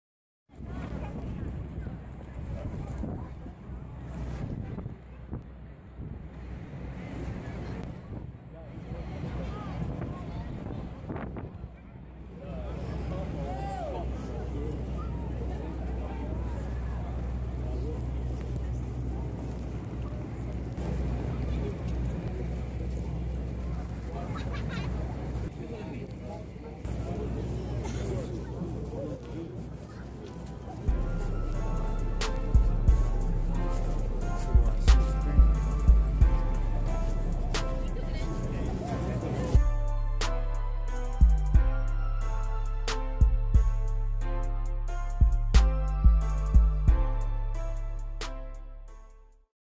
walking down Time Square
short video of me in new York, plus preview for next drawing coming on monday